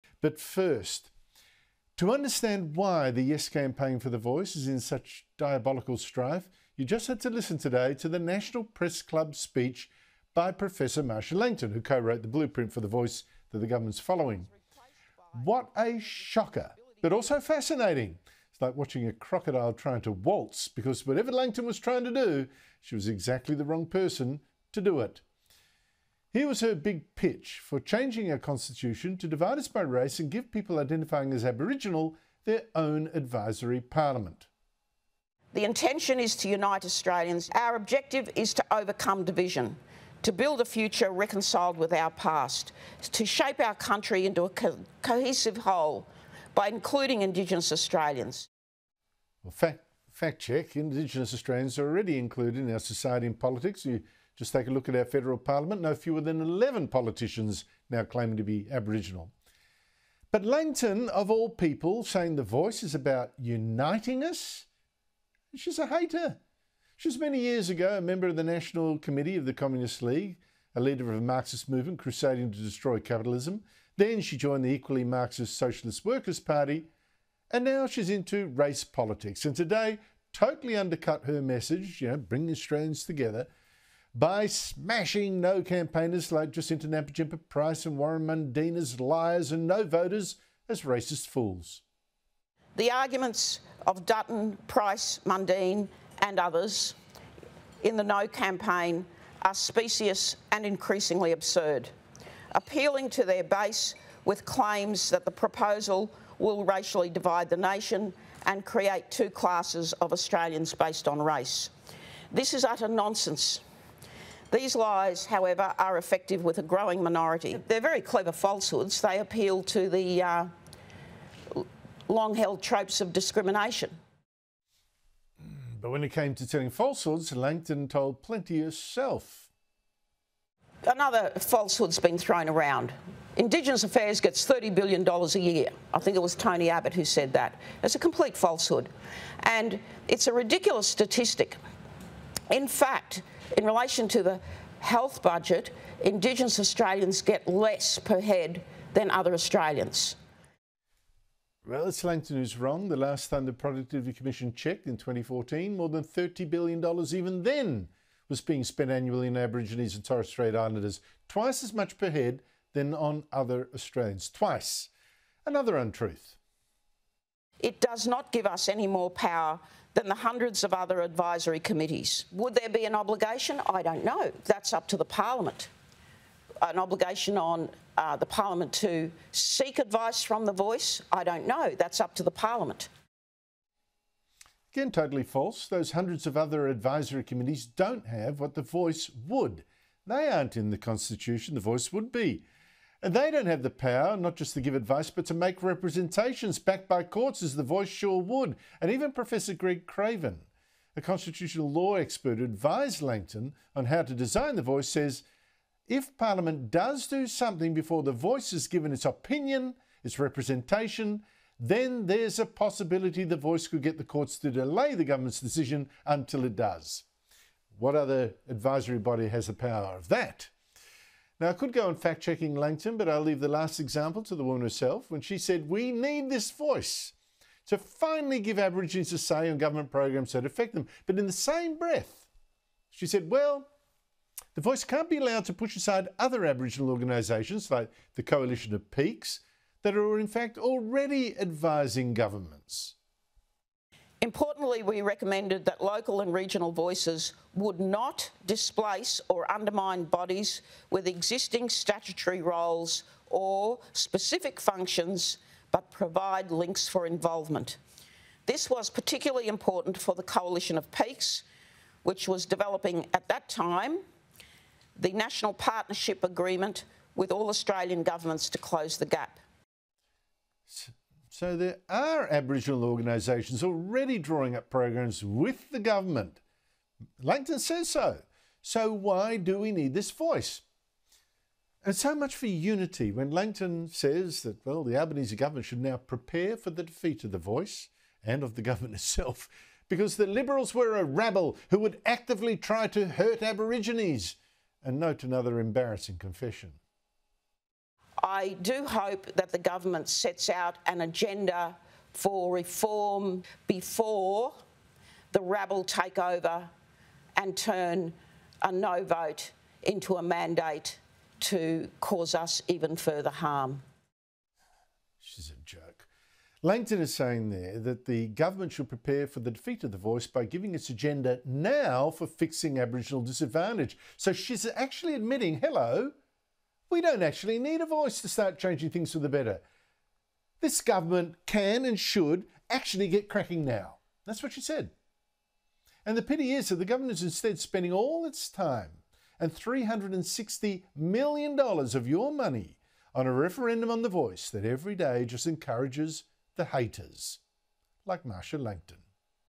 Andrew Bolt fact-checks Marcia Langton’s ‘falsehoods’
Sky News host Andrew Bolt has fact-checked some statements made by co-author of the Calma Langton report, Marcia Langton, during her National Press Club address.

"Whatever Langton was trying to do, she was exactly the wrong person to do it," Mr Bolt said.

"When it came to telling falsehoods, Langton told plenty herself."